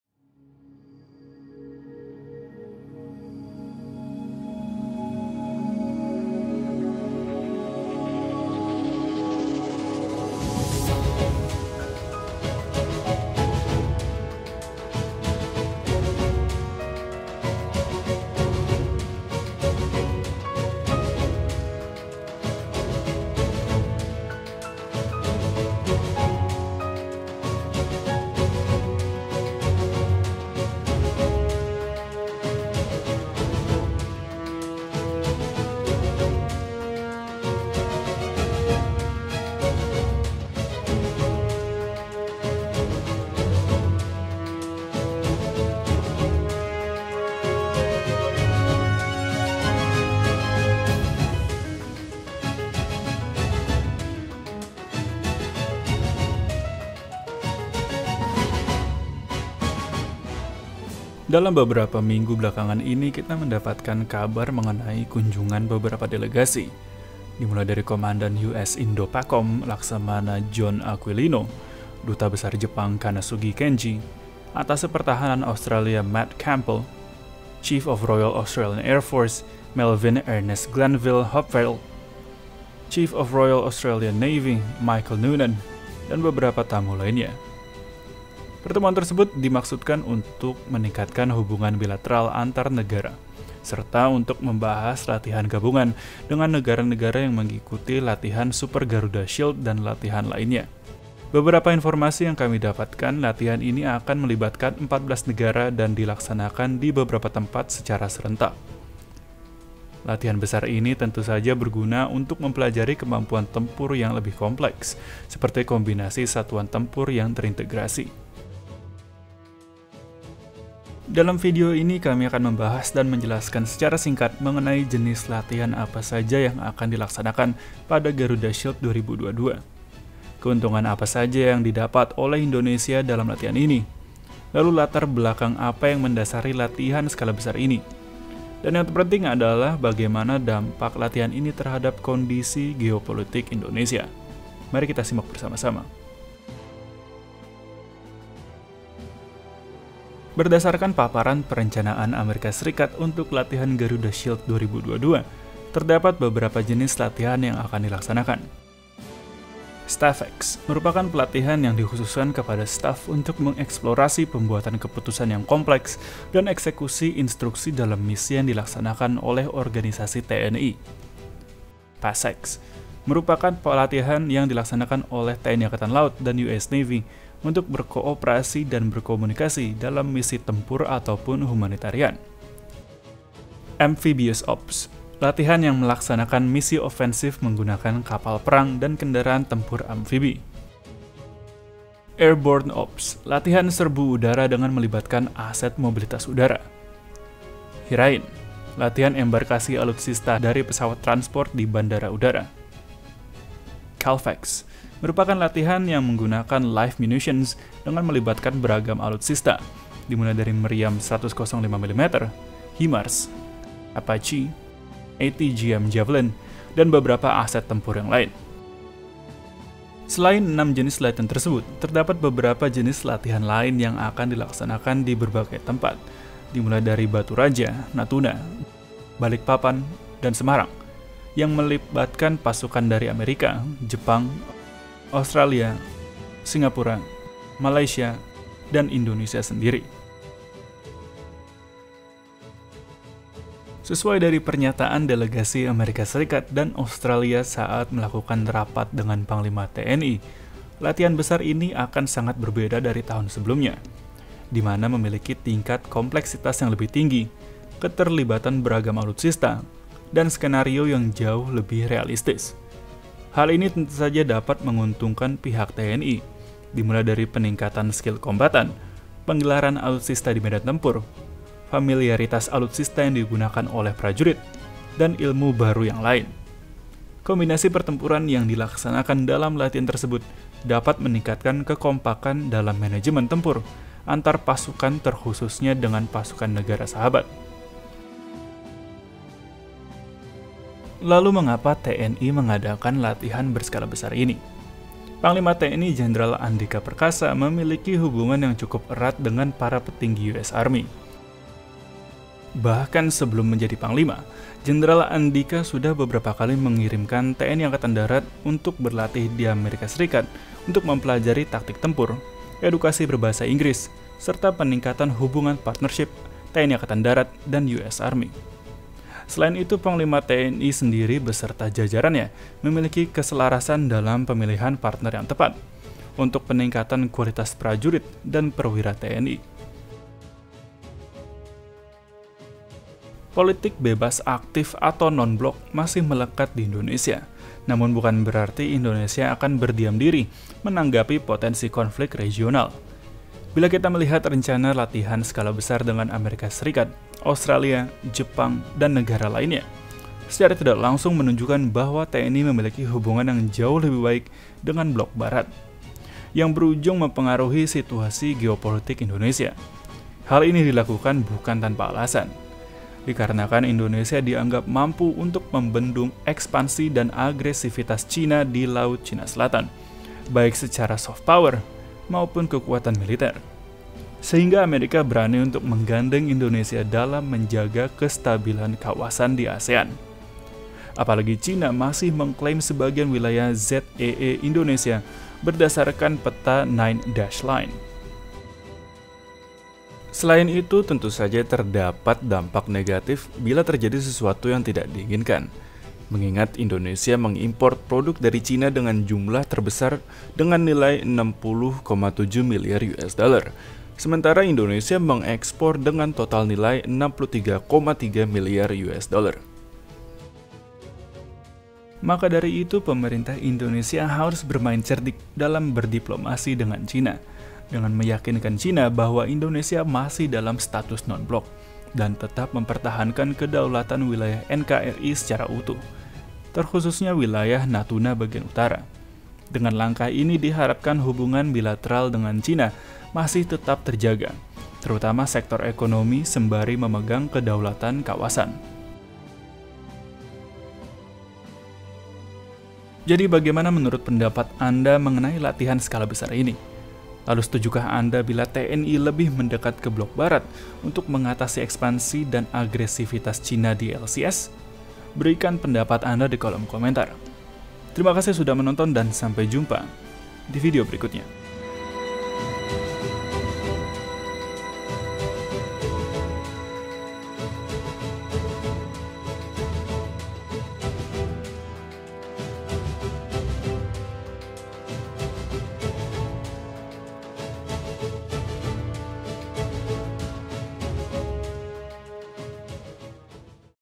14 Negara Ikuti Latihan Gabungan SUPER GARUDA SHIELD, Geopolitik Indonesia Terganggu?
Dalam beberapa minggu belakangan ini kita mendapatkan kabar mengenai kunjungan beberapa delegasi, terkhususnya komandan US INDOPACOM Laksamana John Aquilino. Pertemuan tersebut dimaksudkan untuk meningkatkan hubungan bilateral antar Negara serta untuk membahas latihan gabungan Super Garuda Shield dengan Panglima TNI Jendral Andika Perkasa. Berdasarkan informasi yang kami dapatkan, latihan ini akan melibatkan 14 negara dan dilaksanakan di beberapa tempat secara serentak. Latihan gabungan ini tentu saja berguna untuk mempelajari kemampuan tempur yang lebih kompleks, seperti kombinasi satuan tempur yang terintegrasi.

Tentu saja dalam latihan gabungan ini TNI akan mendapatkan banyak ilmu baru dari negara lainnya, terkhususnya Amerika, Australia, dan Jepang. Akan tetapi latihan Garuda Shield ini memberikan dampak lain dari kondisi geopolitik Indonesia.

Footage:

TNI AD
Sekretariat Presiden
Australian Army
OurSingaporeArmy
Department of Defense Australia
陸上自衛隊 広報チャンネル
Gung Ho Vids
Aiirsource Military
KOARMADA 1
KOMPAS TV
BeritaSatu
Puspen TNI
Jenderal TNI Andika Perkasa
CNBC Indonesia
CNN Indonesia